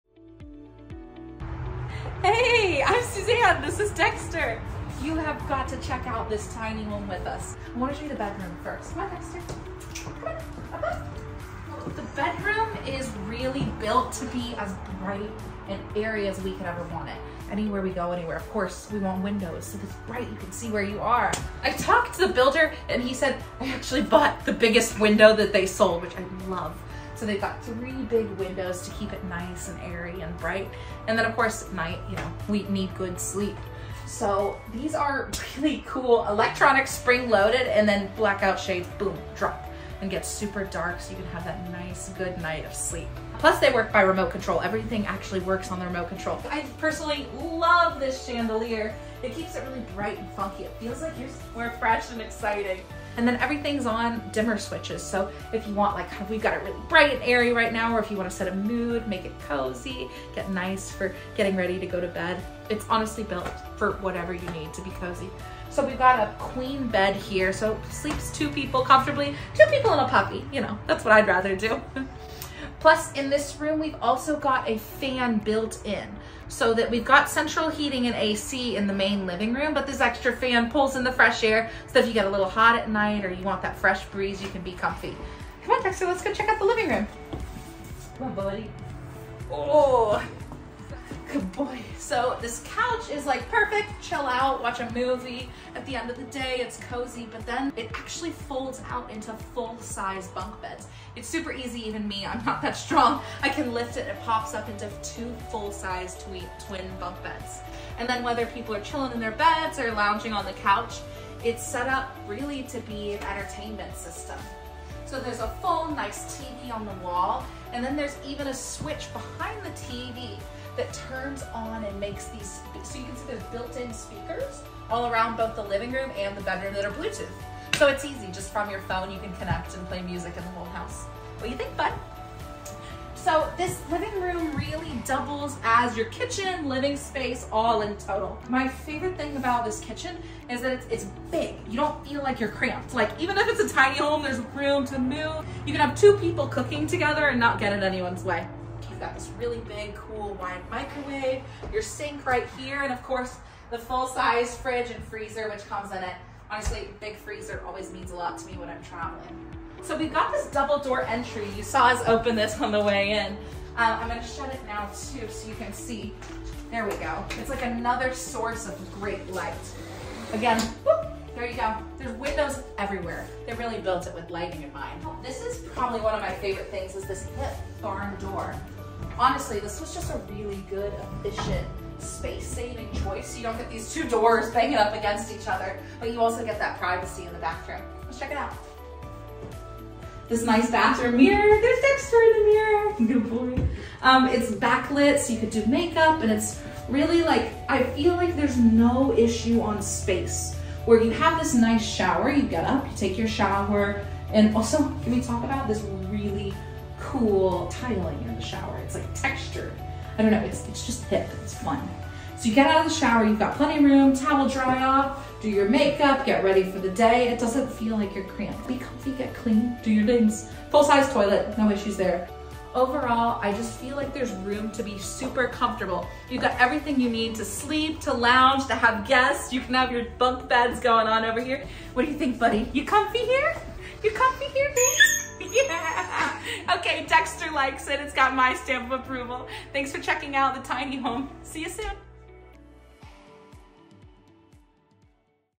32 Ft Long Luxury Tiny Home For Sale in Raleigh, North Carolina Tiny House Tour
This Is a built and ready new Tiny Home!

Video tour available. This Tiny home is ready to be your next Airbnb income generator, hunting cabin, or vacation home! Built with the best materials out there: steel framed, Trailer Made trailer, hydrophobic spray foam insulation, VaproShield Frog-skin waterproofing, LP Smartside siding, and so much more. Ten-foot interior ceilings! The living area has a Luonto Elevate Bunk Bed Sofa Sleeper that transitions from couch to twin bunk bed in 10 seconds. The back bedroom features a queen bed, 6’6” tall casement windows, dimmable chandelier and cup lights, and electric blinds. It has a full-size tile bathroom, including a washer/dryer nook. Shoot me a message to schedule an in-person tour!

Interior Ceiling Height: 10 Feet

Ground to Roof Height : 13 Feet

Estimated Square Feet: 256 Sq Ft

Width x Length: 8 ft wide x 32 ft long (36ft including trailer tongue)

Interior Wall Details: 8” Nickel Gap Shiplap

Flooring Details: Engineered Hardwood

Kitchen Countertop & Cabinet Details: Euro Style Cabinets with quartz counters

Kitchen Sink Details: Full size single bowl

Stove & Exhaust Vent Details: Two burner induction stove

Washer & Dryer Details: Plumbed for RV style washer/dryer

Lighting Details: All lighting is LED and living areas are all on dimmer switches

Bathroom Details

Shower: 36”x 6’ Tile shower shower

Toilet Type: A Traditional Flushing Toilet – wired for a bidet

Structure Details

Number of Windows: 6 Windows

Structure Framing Material Details: Steel Framed

Roof Material Details: Crinkle Finish Standing Seam Metal Roof

Plumbing Details: all plumbing is PEX and PVC piping

Sewer hookup Details: sewer hookup is on the side of the trailer – there are no tanks

Insulation Location & R-Value Details: Hydrophobic open-cell spray foam

Trailer Details

Trailer Brand & Hitch Size: Trailer Made Trailers – Hitch size is 2 5/16

Trailer Brake Lights: Yes – Functional

Trailer Breaks: Trailer is equipped with breaks